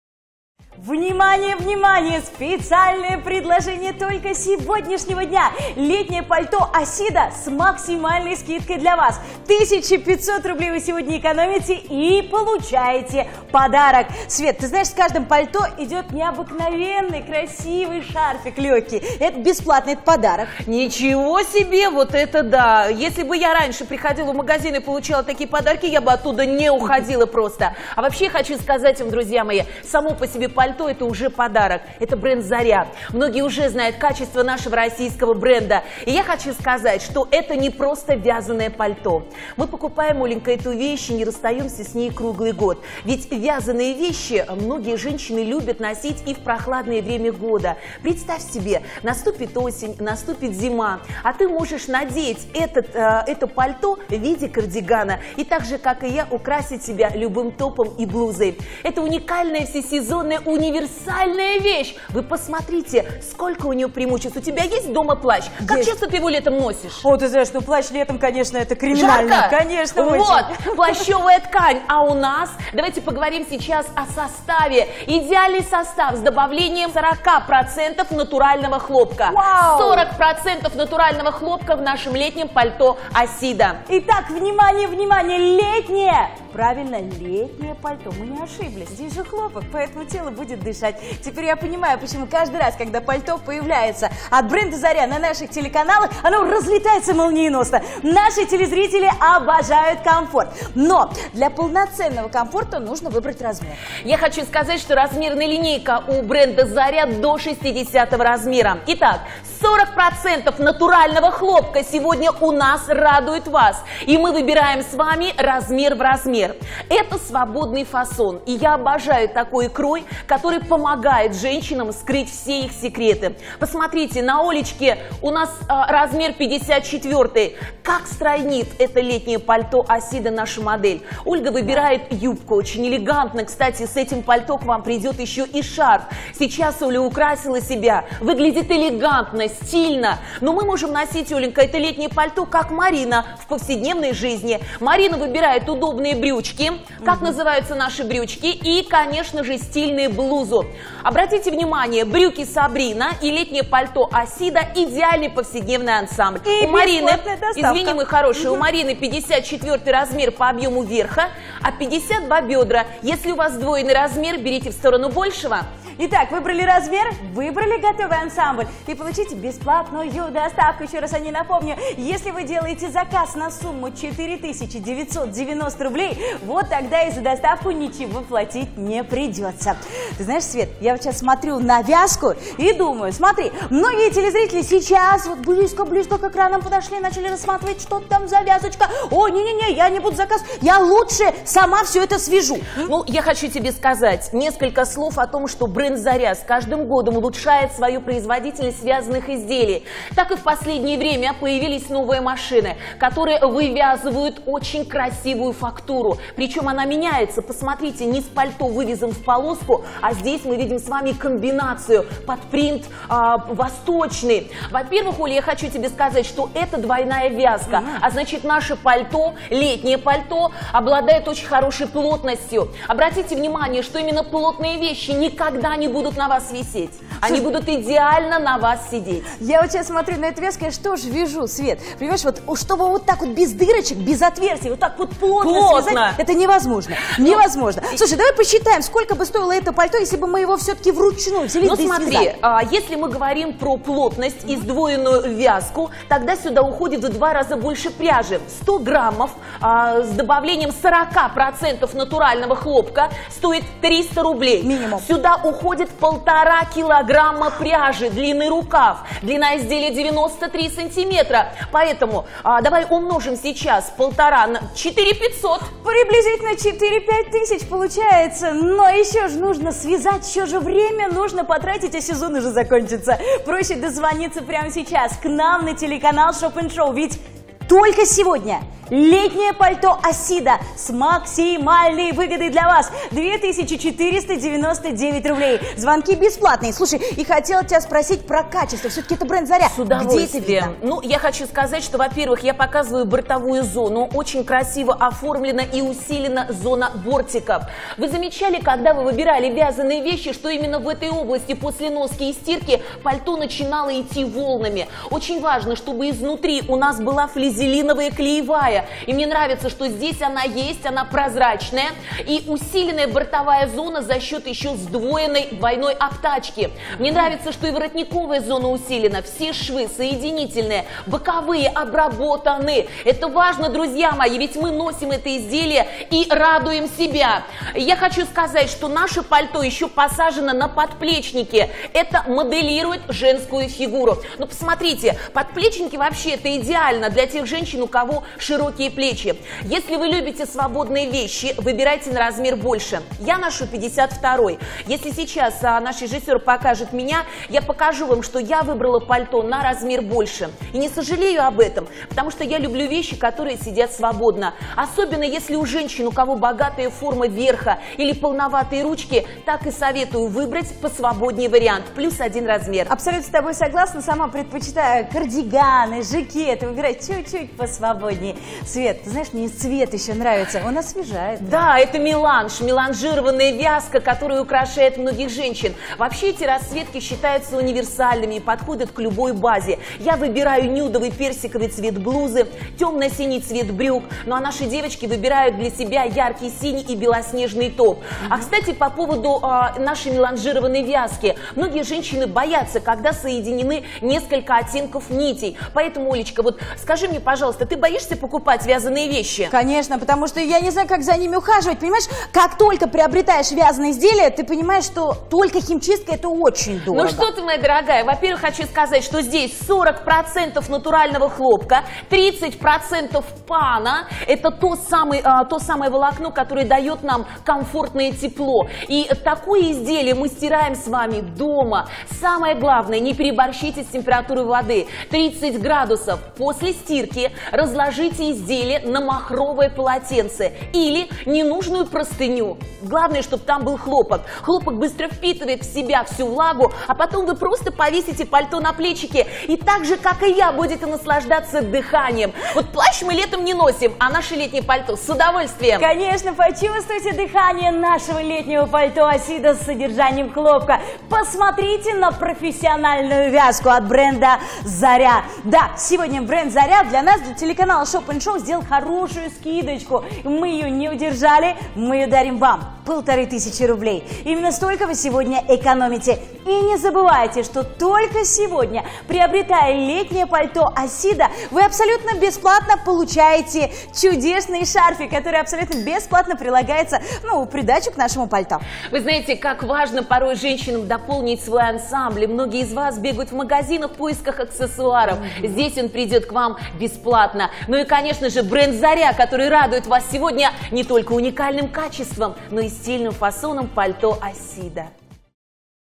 Летнее пальто Асида
Обзор на товар летнее пальто "Асида"